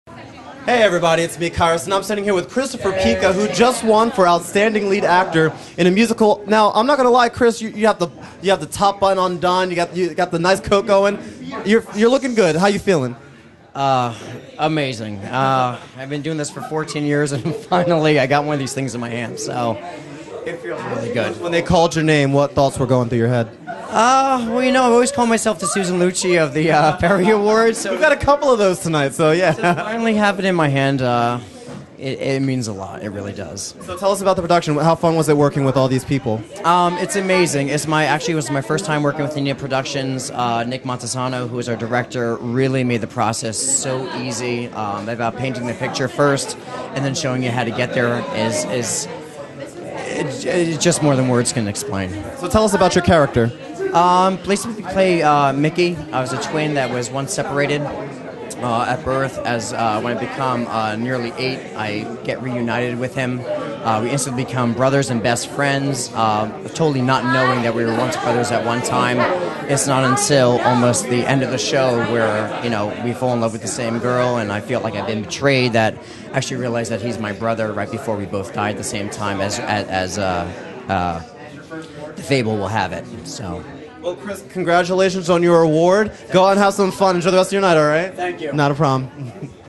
Perry Award 2009 - Interview 44
Red carpet host Kyrus Westcott interviews Christopher Pica winner of outstanding Lead Actor in a Musical at the 2009 Perry Awards